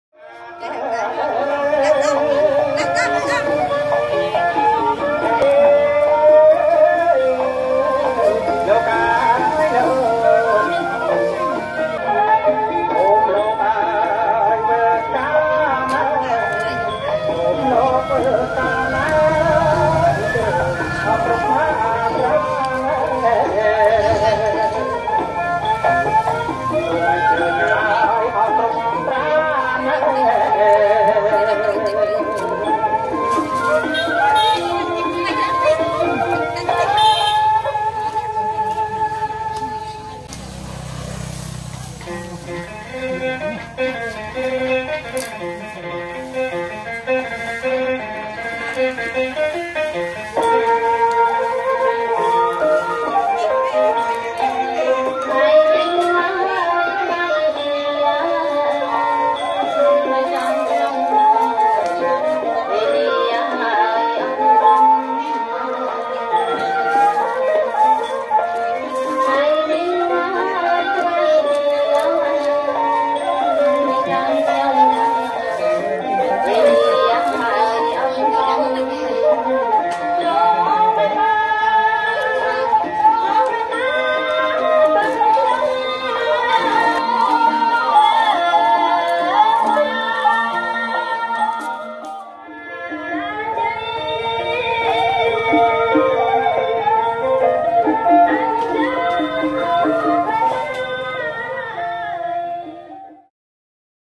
Preparing Fish for a Cambodian Wedding Feast
Stuffing Fish with a stick of lemon grass and a kaffir lime leaf...
Wedding food on this day, 17 Nov 2019, in the Cambodian countryside.